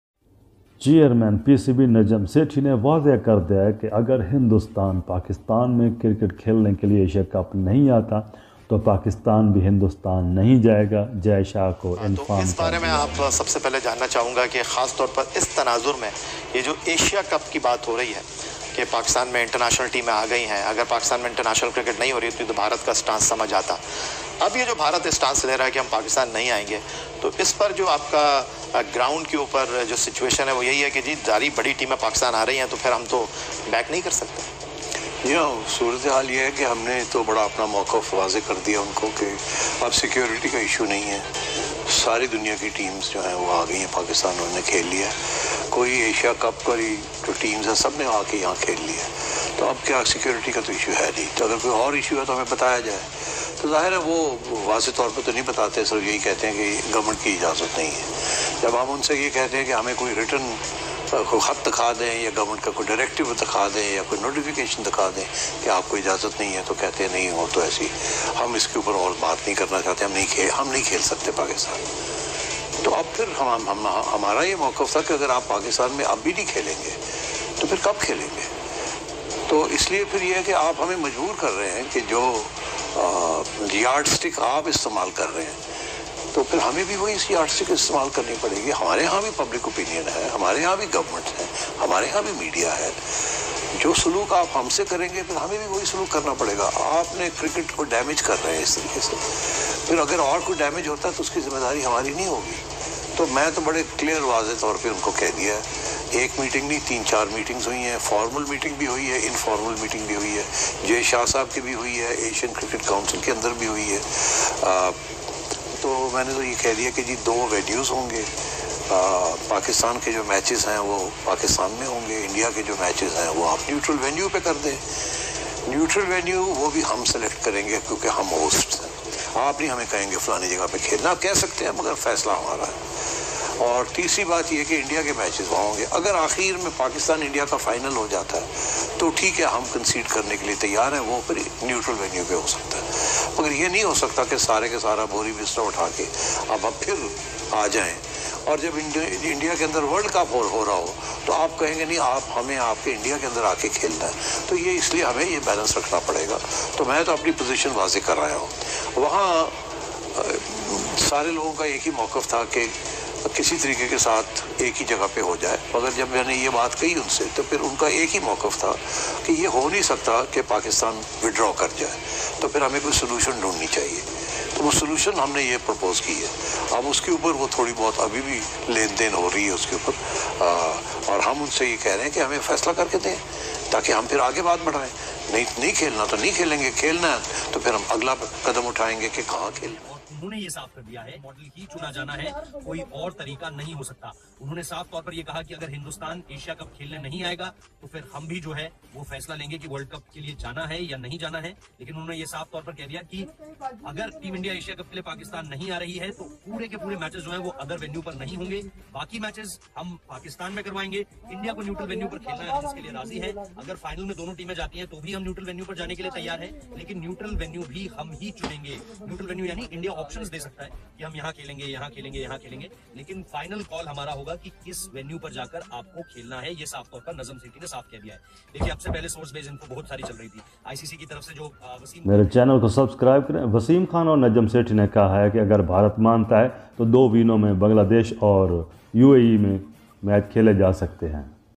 najam sethi speech on Asia cup indian team well came pakistan or ?
it's choice of Indian team came pakistan its better and not come no problem. we well not reach Indian for world cup.